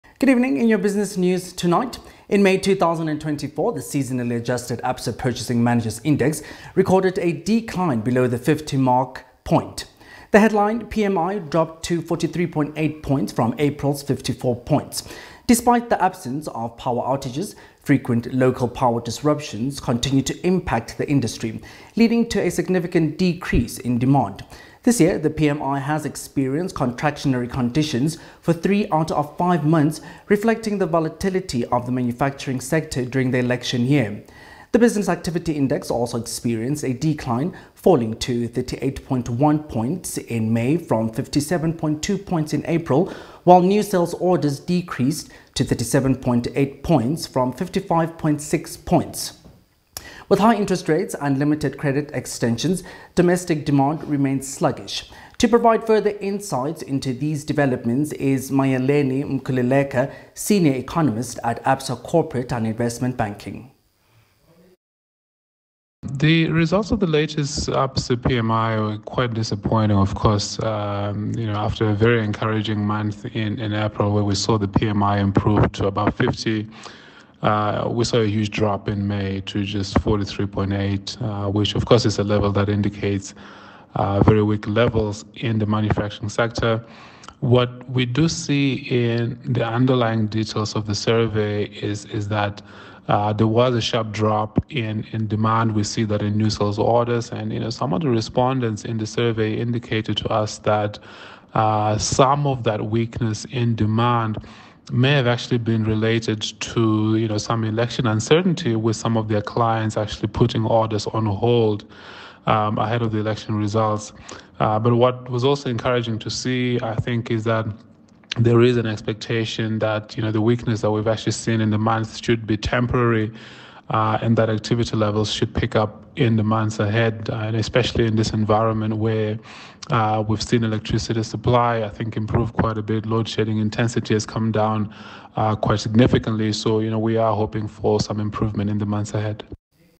SOWETO TV NEWS | ABSA PMI DROPS TO 43.8 AMID ELECTION UNCERTAINTY AND WEAK DEMAND
Absa Purchasing Managers' Index drops to 43.8 amid election uncertainty and weak demand.